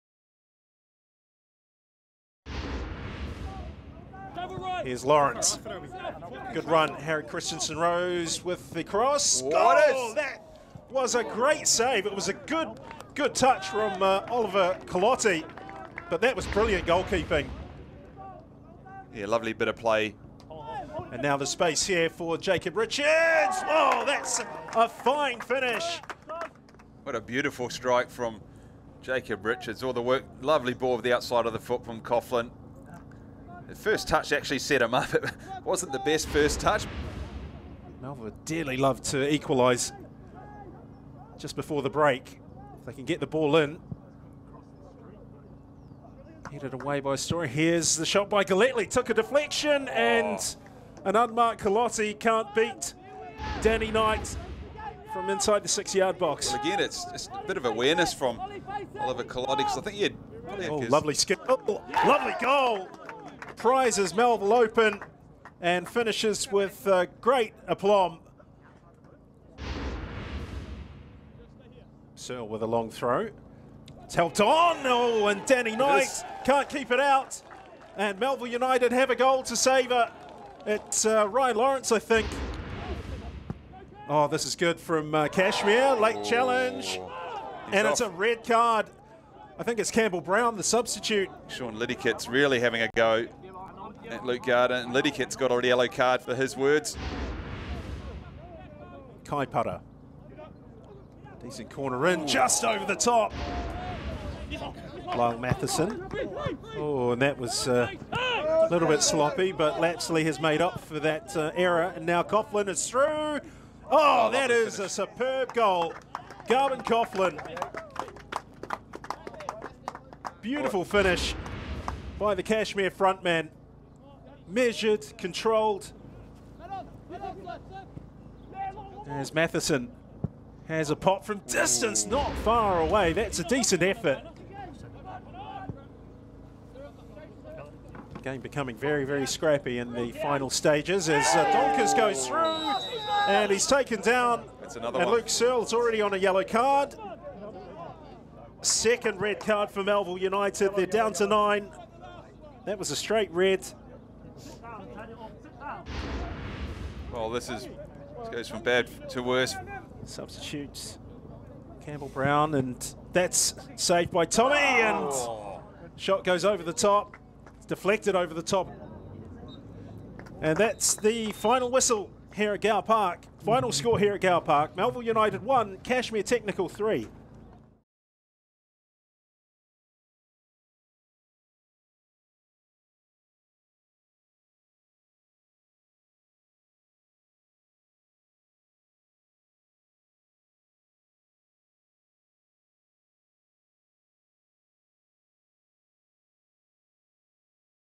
Melville United v Cashmere Technical | Highlights | National League Championship
Highlights from Melville United vs Cashmere Technical in Round 4 of the men's National League Championship.

Highlights provided by Sky Sport NZ.